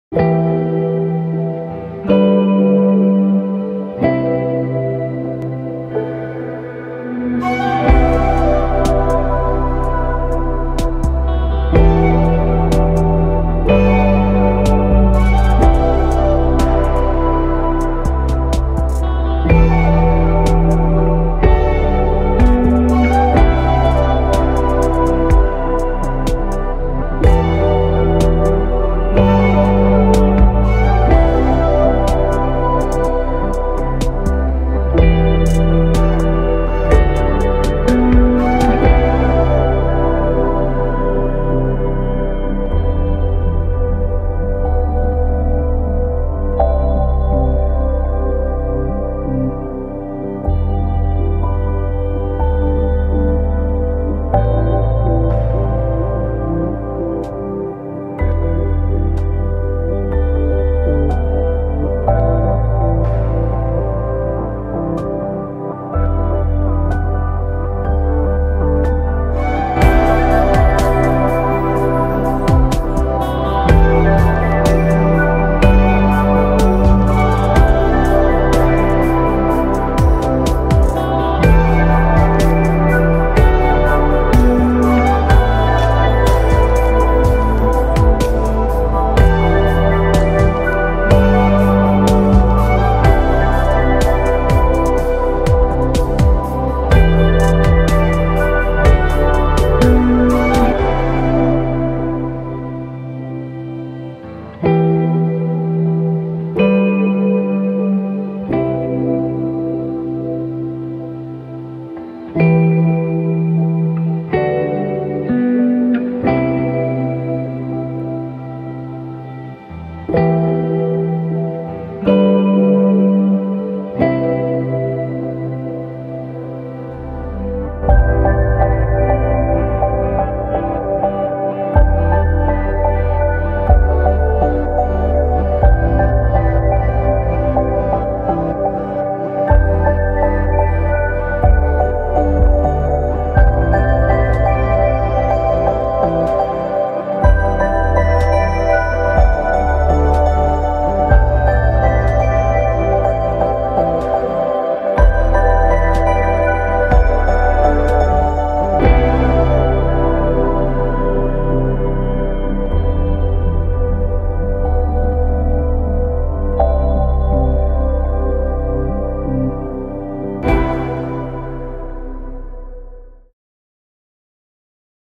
Doctors Day Special, a story for children
A story about friendship, and a doctor's invaluable help in solving our problems.